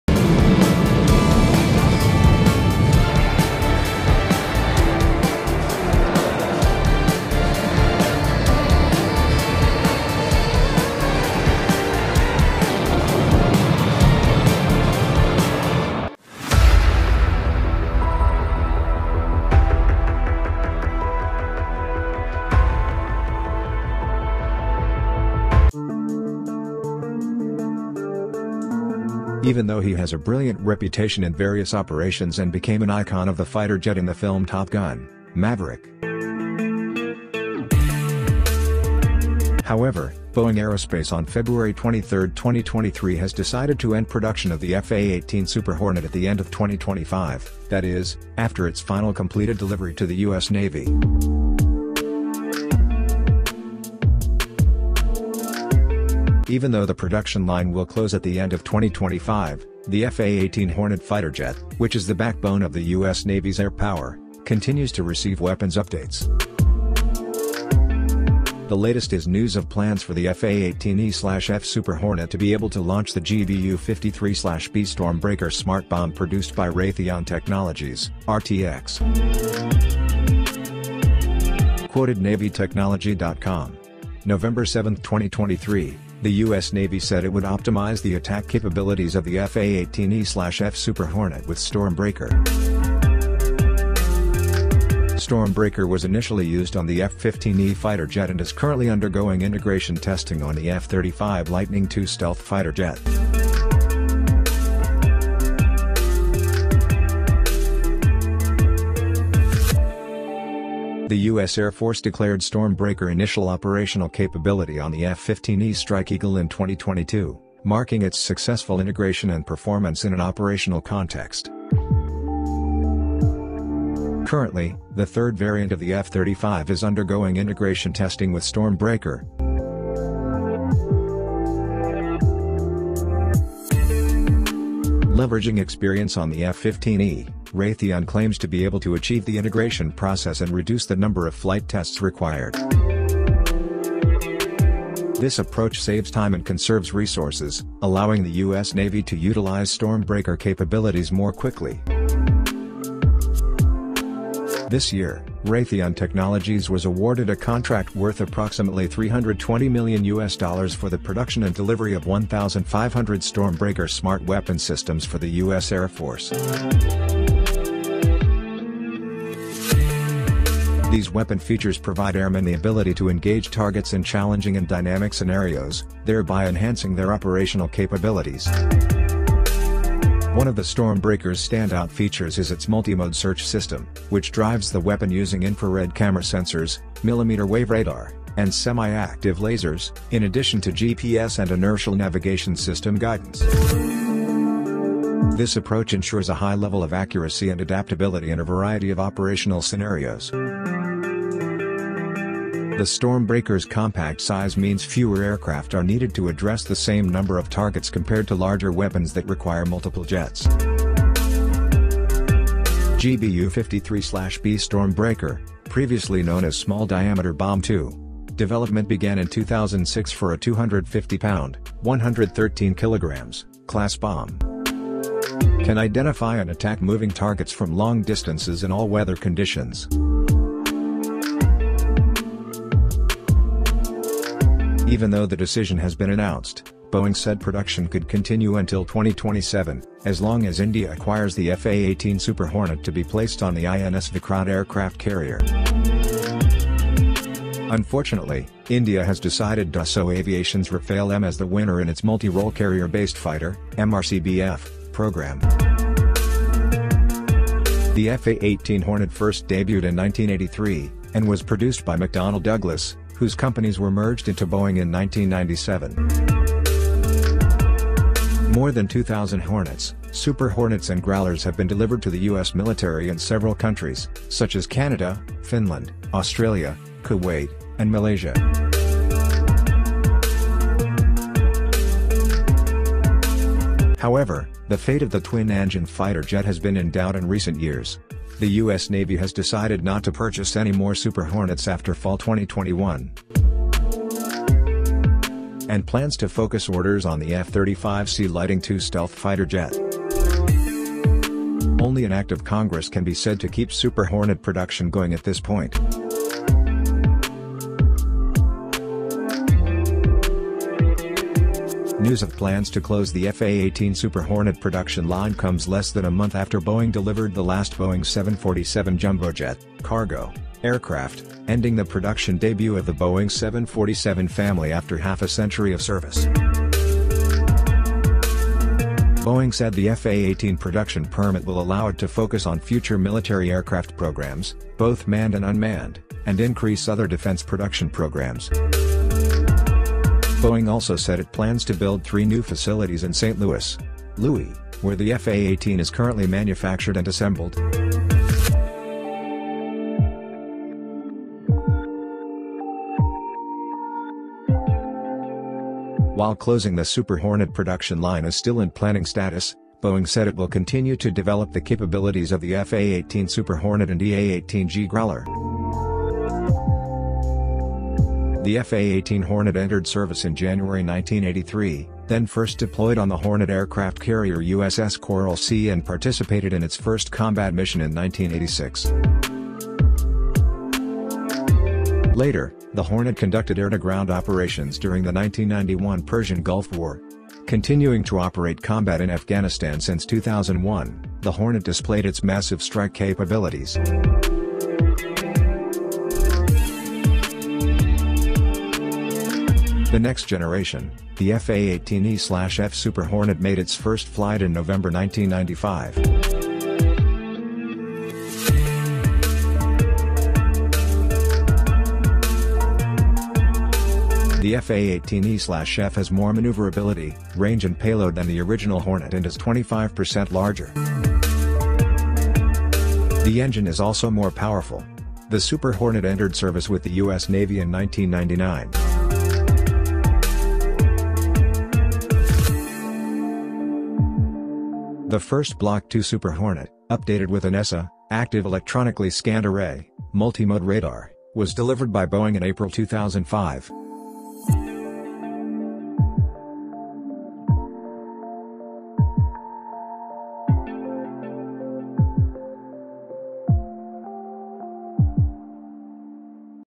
After 4 Decades, the US Navy Integrates the F/A-18 Super Hornet With the StormBreaker
Most recently, Boeing delivered its first F/A-18E/F Super Hornet Block III to the US Navy in September 2021. The manufacturer describes this aircraft as the most advanced version of the Super Hornet and surpassing the capabilities of fourth-generation fighters. Happy following and Thank you for your visit, I like you 🙏🙏👍